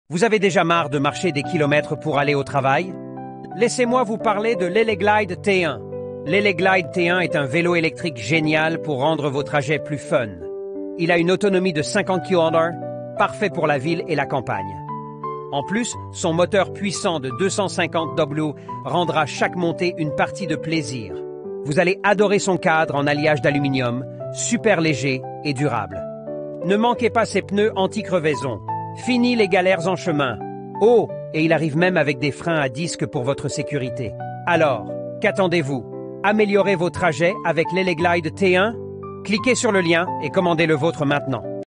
Eleglide T1 FR 1
Vélo électrique de trekking électrique Eleglide T1 27,5 pouces Pneus 36V 13Ah Batterie 250W Moteur Shimano 7 vitesses, vitesse maximale 25 km/h Portée maximale 100 km IPX4 étanche double frein à disque - Noir

Points forts
Moteur puissant et grande batterie
Pneus CST 27,5"
Engrenage Shimano à 7 vitesses
Suspension hydraulique avec verrouillage
3 modes de conduite et 5 niveaux de vitesse
Affichage intelligent